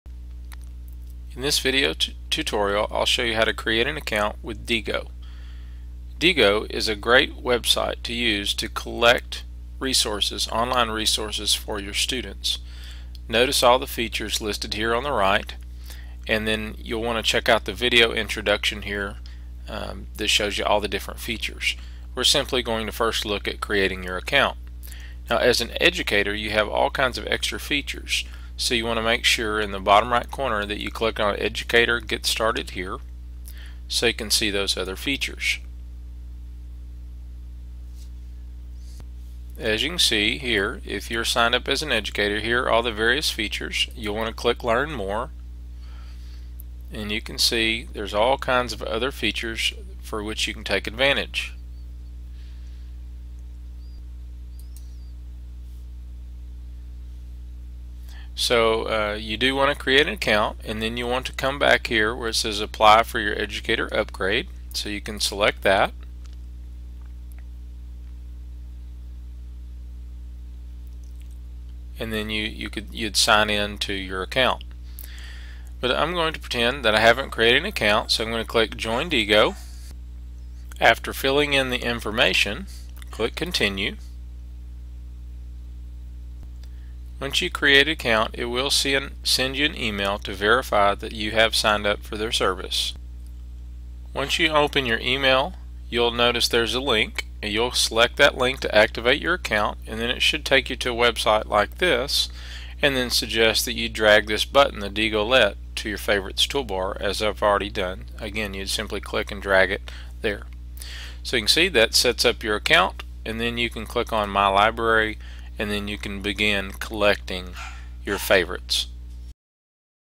Diigo: Create an Account
Learn how to setup your Diigo account for the first time